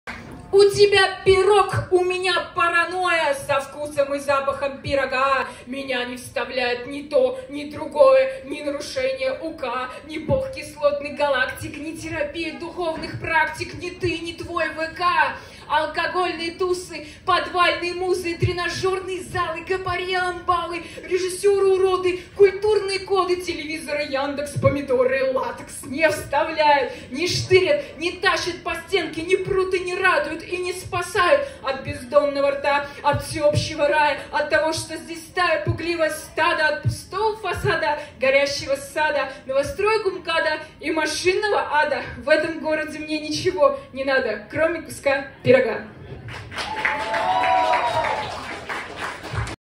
Паранойя – Алёна Синица – (ЛитПон #311) – 23.05.22 – Китайский Лётчик Джао Да (г. Москва).
«Сегодня ЛитПон похож на рок-фестиваль: сидящие на полу зрители запивают хот-доги пивом и благодарно наблюдают, как стираются границы между поэзией, комедийным стендапом, хип-хопом и театром. Закулисы производят не меньшее впечатление: десятки гримерок, в которых поэты переодеваются в эксцентричные наряды, фотографируются, пьют виски, шутят, курят, орут и смеются...» © журнал «Афиша».
Алёна Синица на сцене летней веранды клуба Китайский Лётчик Джао Да на время проведения «Литературного Понедельника» у Арса-Пегаса №311 – 23 мая 2022 года. 🕯💖✍️